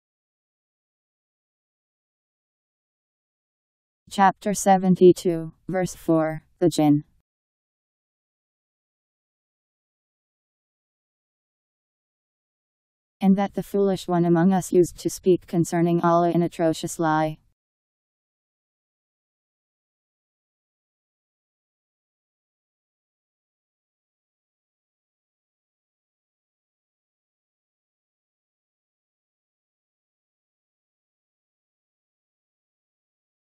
Quran The Jinn 72:4
Video shows Quran The Jinn Chapter 72 Verse 4: And that the foolish one among us used to speak concerning Allah an atrocious lie. Powered by MaryTTS. Holy Qur'an 72:4 Surah : Ayah